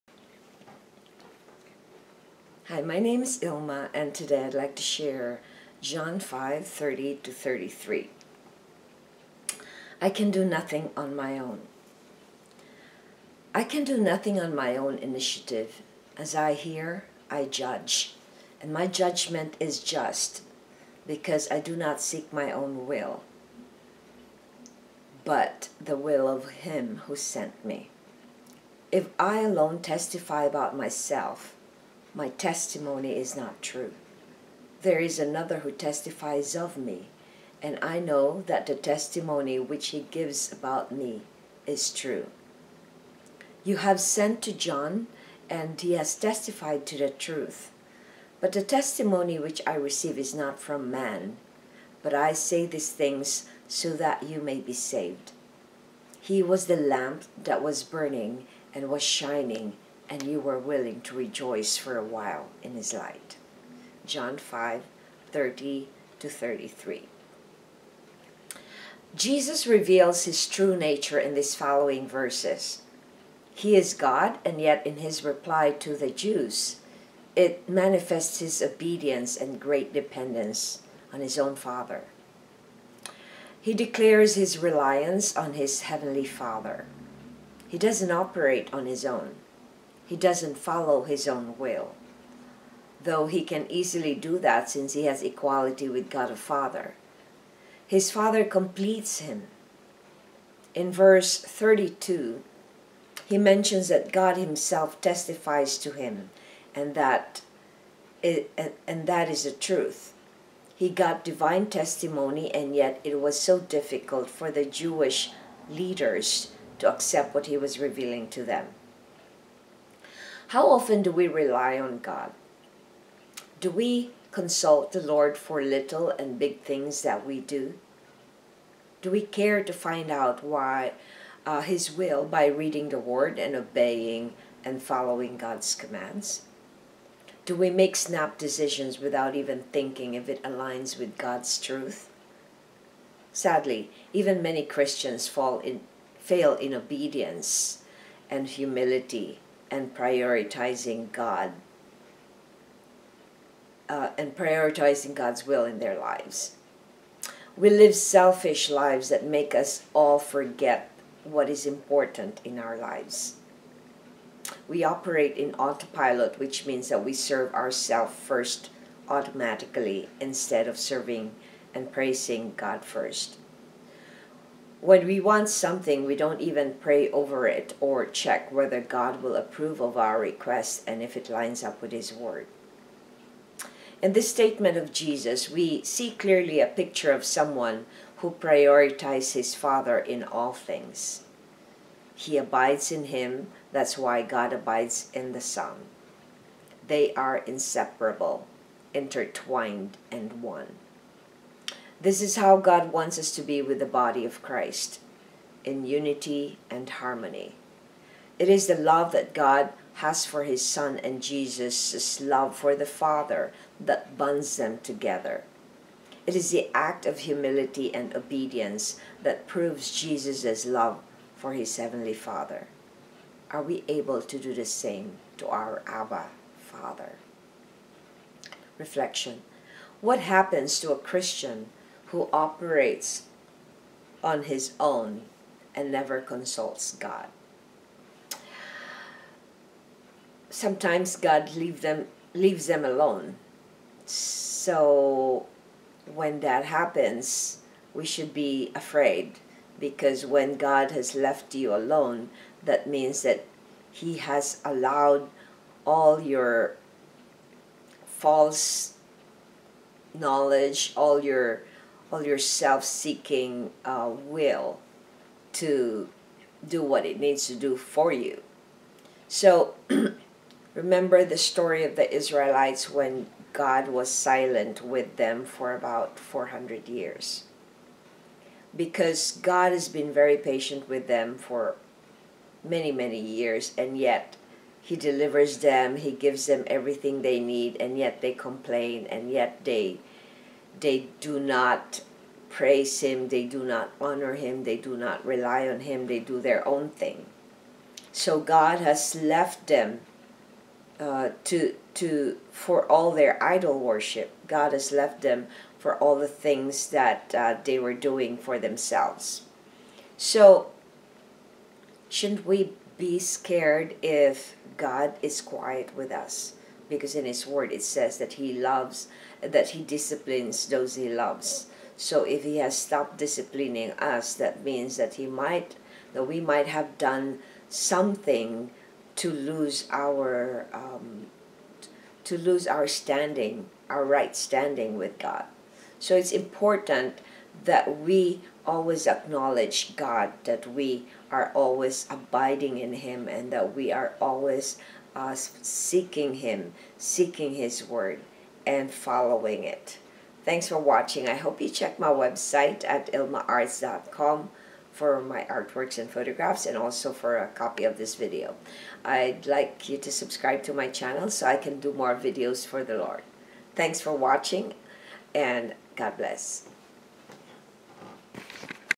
JOHN 5 I CAN DO NOTHING ON MY OWN ILMA'S VLOG
June 27
________________________________________I CAN DO NOTHING ON MY OWN
30 “I can do nothing on My own initiative. As I hear, I judge; and My judgment is just, because I do not seek My own will, but the will of Him who sent Me.31 “If I alone testify about Myself, My testimony is not true. 32 There is another who testifies of Me, and I know that the testimony which He gives about Me is true. 33 You have sent to John, and he has testified to the truth. 34 But the testimony which I receive is not from man, but I say these things so that you may be saved. 35 He was the lamp that was burning and was shining and you were willing to rejoice for a while in his light.– John 5:30-33
Jesus reveals his true nature in these following verses. He is God and yet in his reply to the Jews, it manifests his obedience and great dependence on his own Father. He declares his reliance on his Heavenly Father. He doesn’t operate on his own. He doesn’t follow His own will, though he can easily do that since He has equality with God the Father. His Father completes him. In verse 32, he mentions that God himself testifies to Him and that is the truth. He got divine testimony and yet it was so difficult for the Jewish leaders to accept what he was revealing to them.
How often do we rely on God? Do we consult the Lord for little and big things that we do? Do we care to find out his will by reading the word and obeying and following God’s commands? Do we make snap decisions without even thinking if it aligns with God’s truth? Sadly, even many Christians fail in obedience and humility and prioritizing God’s will in their lives. We live selfish lives that make us all forget what is important in our lives. We operate in auto-pilot which means that we serve our self first automatically instead of serving and praising God first. When we want something, we don’t even pray over it or check whether God will approve of our request and if it lines up with his word.
In this statement of Jesus, we see clearly a picture of someone who prioritizes his Father in all things. He abides in Him that’s why God abides in the Son. They are inseparable, intertwined and one. This is how God wants us to be with the body of Christ, in unity and harmony.  It is the love that God has for His Son and Jesus’ love for the Father that bonds them together. It is the act of humility and obedience that proves Jesus’ love for his heavenly Father. Are we able to do the same to our Abba Father?
REFLECTION
• What happens to a Christian who operates on his own and never consults God?